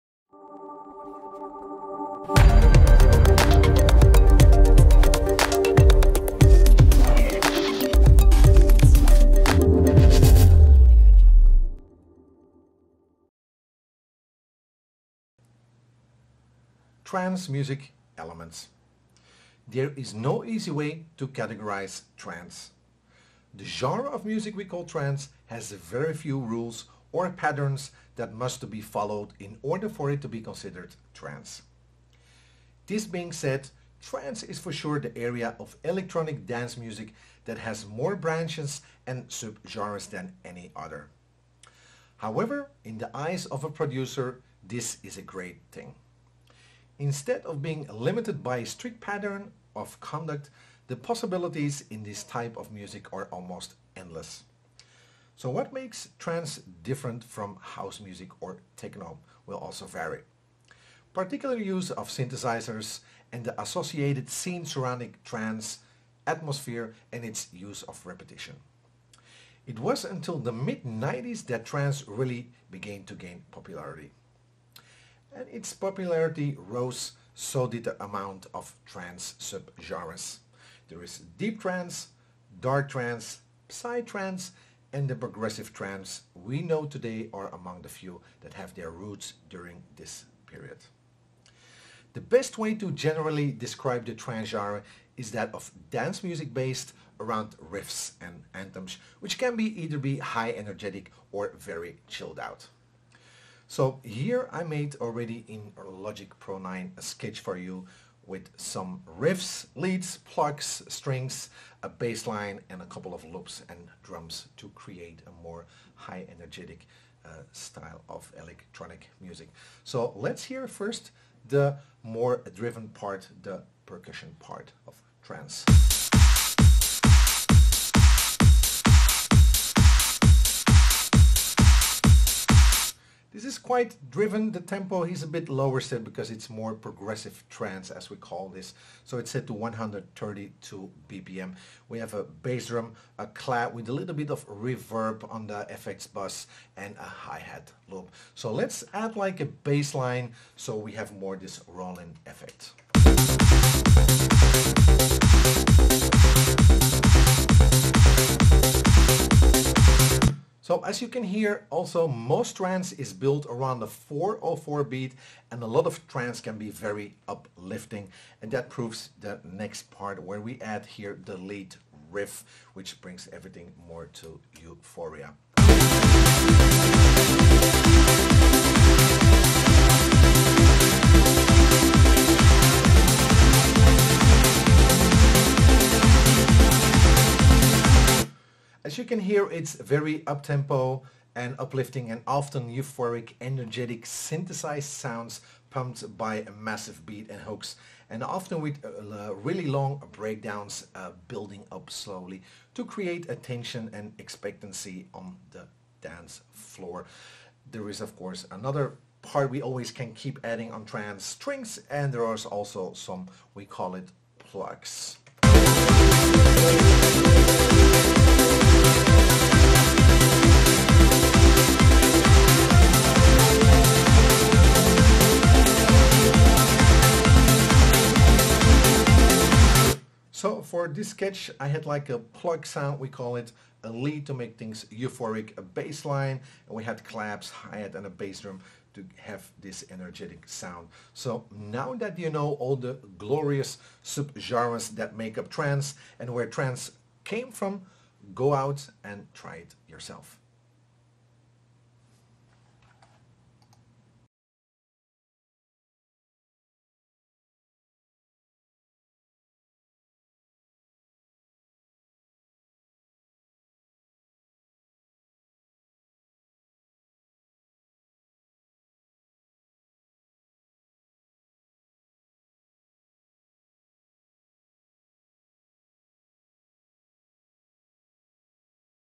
musicboxx presents... Using TRANCE ELEMENTS in Logic Pro with MIKE DIERICKX
musicboxx is proud to have MIKE DIERICKX outline some of the basics of using TRANCE ELEMENTS in Logic Pro..  His record is extensive producing and remixing for many artists such as Moby, Sinead O'Connor, 2 Unlimited, Armin van Buuren, Ferry Corsten just to name a few.

This video has been specifically designed for the novice electronic music producer.  Mike is using Logic pro 9 in this video.  However the video outlines principles that can be applied to any other PLATFORMS/SOFTWARE or DAW (Digital Audio Workstation).

Focal points he will be covering:
-Trance is differentiated by the particular use of synthesizers, associated synths, atmosphere and its use of repetition.
-Includes riffs, leads, plugs, strings, bassline, loops & drums to create a high energy
-Strings bring more depth to the track
-Most Trance has the 4/4 beat
-The lead riff is key, brings energy
-Breakdowns help create tension and expectancy on the dancefloor